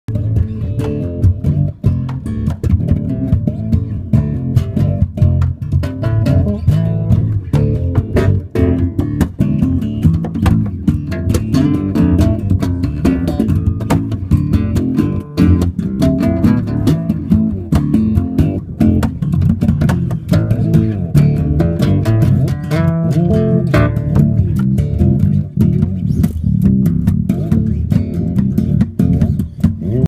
Ryan Martinie, Leo Maksimov, Dmitry Lisenko - Bass Trio Jam
Trio Bass Jam!!! During the Warwick Bass Camp 2016 we were jamming on the Framus & Warwick roof.
With Ryan Martinie from Mudvayne & Leonid Maksimov from Linda!